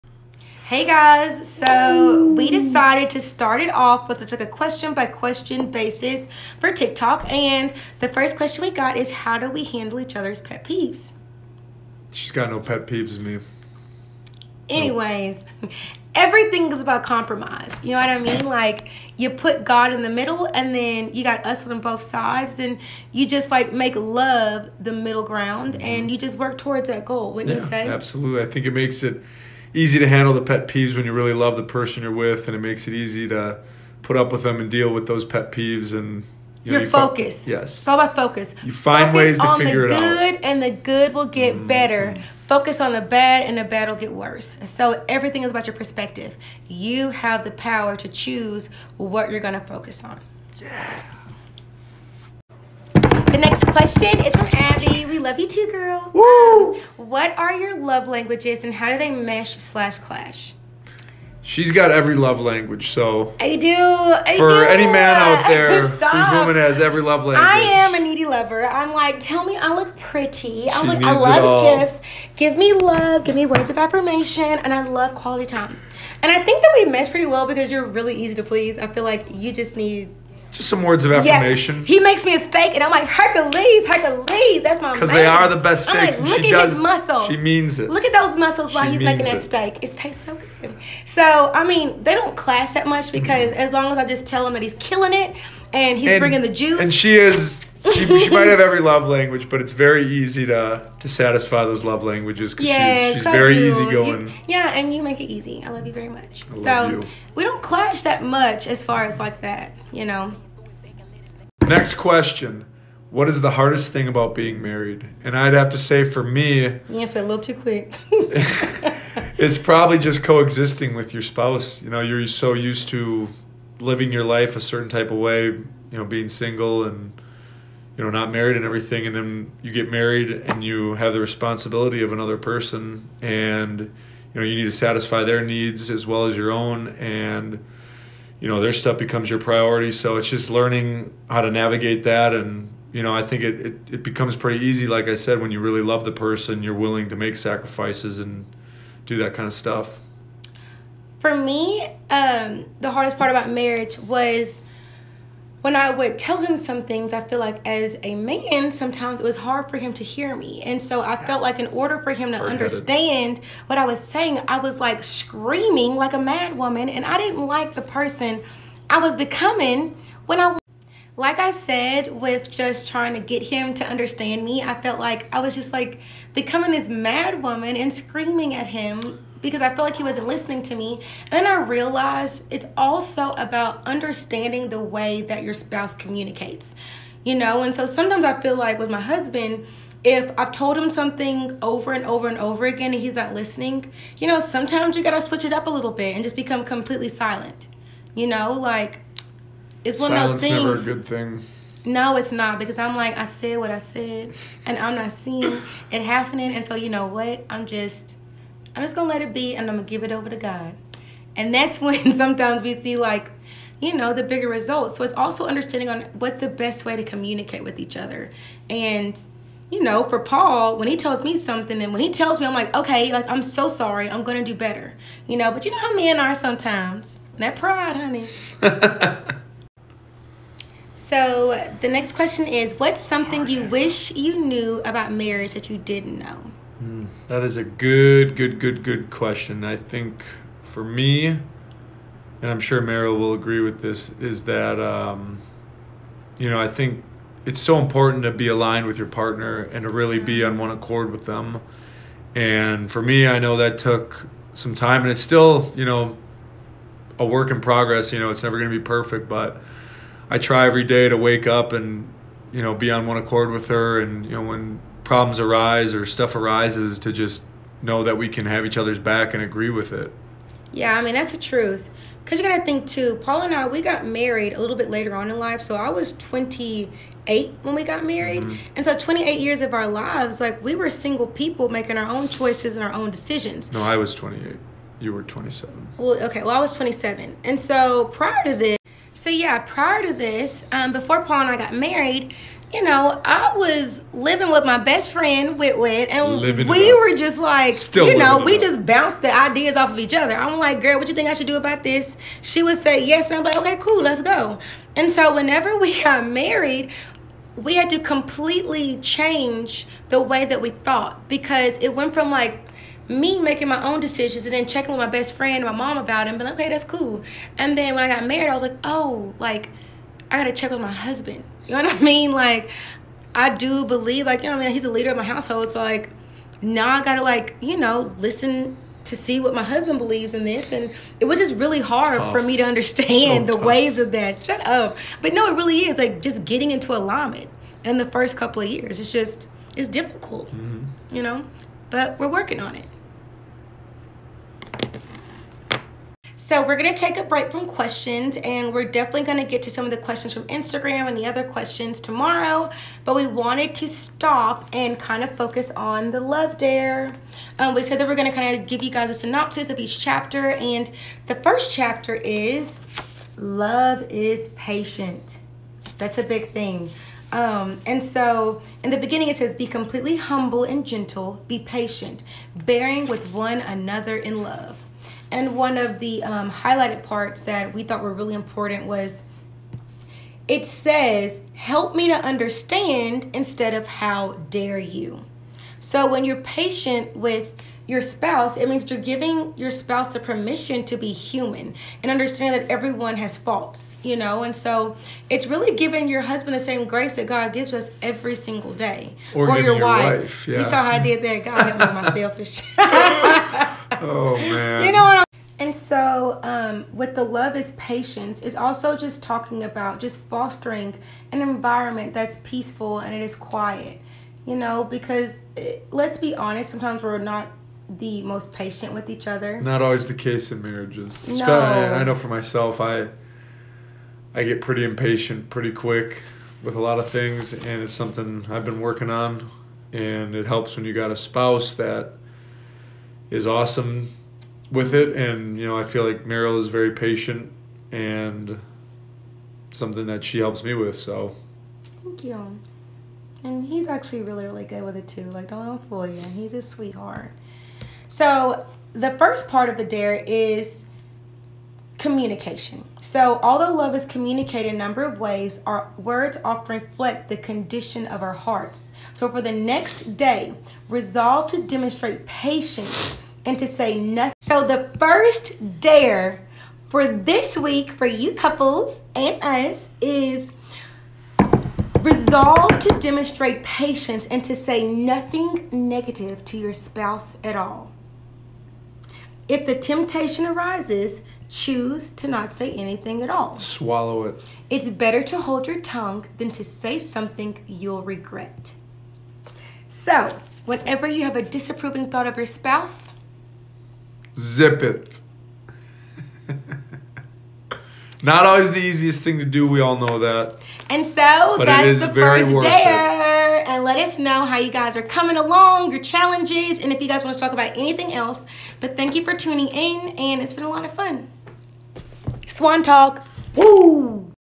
Swan Talk : Episode 1
Swan Talk : Episode 1 In Part One we answer a couple of y’all’s questions and propose the 1st Dare!!!
The Dare: Say nothing negative to your spouse 🤐🤐🤐🤐. We will try and do these weekly!! Keep your questions coming and share with us how you’re doing with the challenge!! (And remember) Love is a Journey, not a Destination!! So come along with us @pswan33
*We will get to all the questions throughout the series. 🦢*